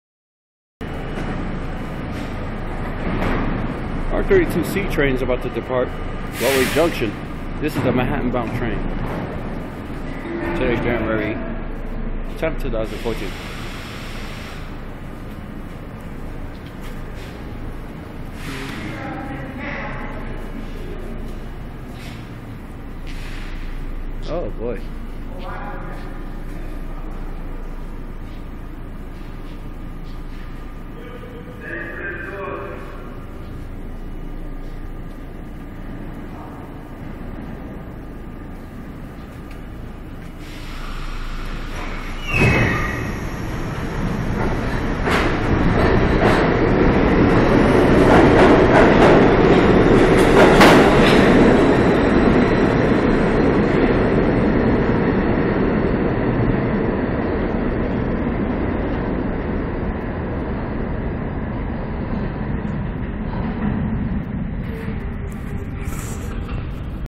Manhattan Bound R32 C Train Departs Broadway Junction
Title says it all. January 10th 2014